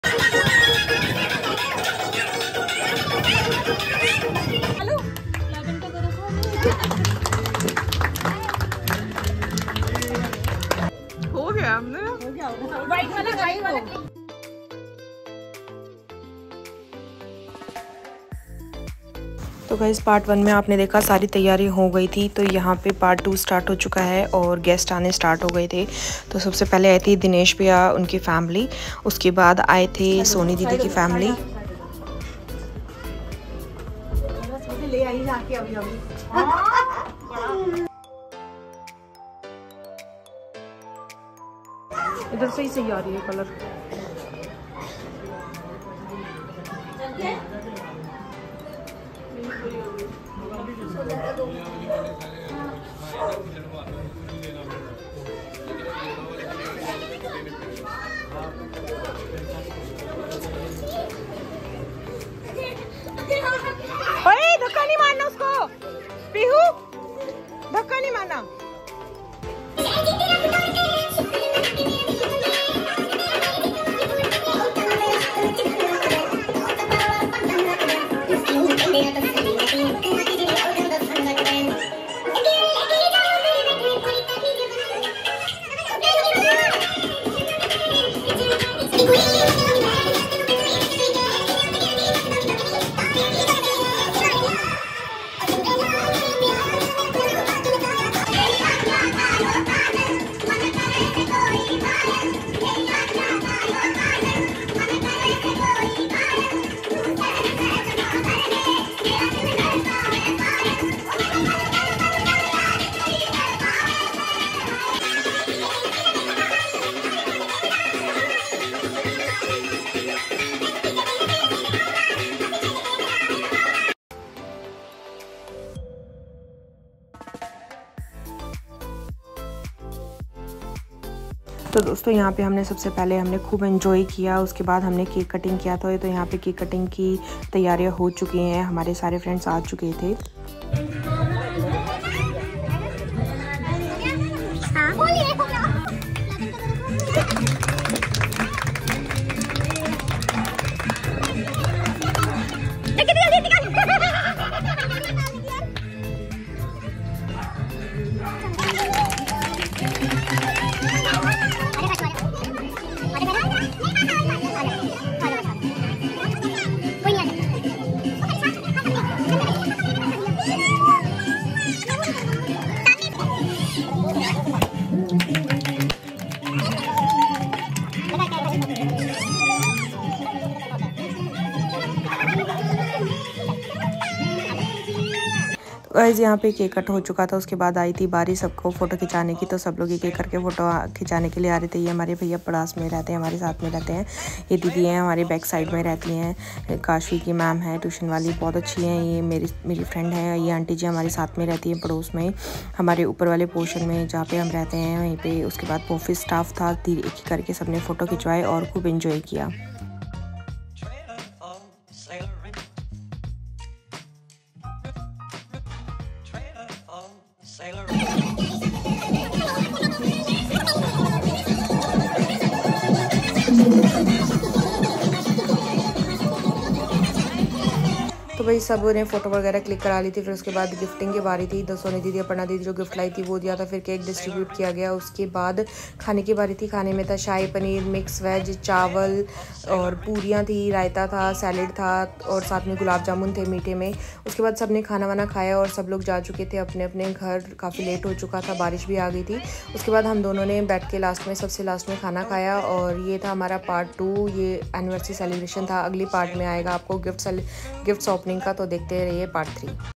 Part-2 Anniversary celebration 🎉|| 7th सालगिरह || देखो वरमाला पहनते वक्त क्या हुआ 😂
Is vlog mein hamari 7th wedding anniversary celebration dikhaya gaya hai